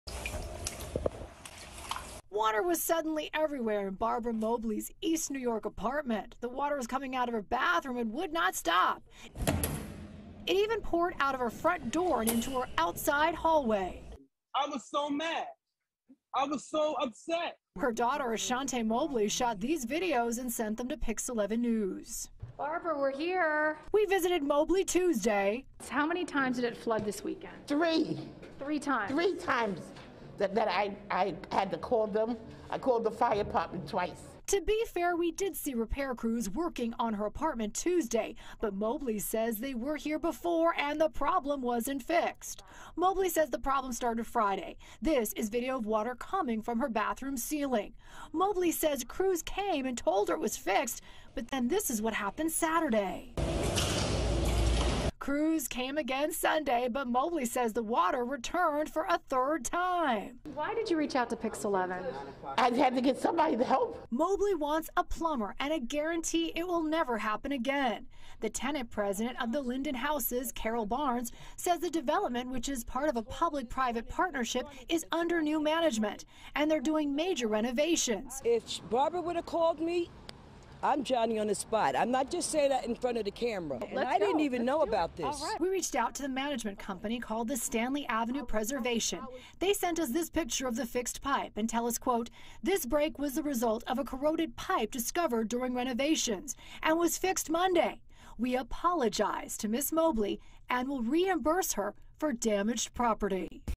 Brooklyn great-grandma needs home repairs after apartment floods
This weekend was a nightmare for a Brooklyn great-grandmother when water suddenly came out from her bathroom and would not stop, even pouring out her front door and into the hallway.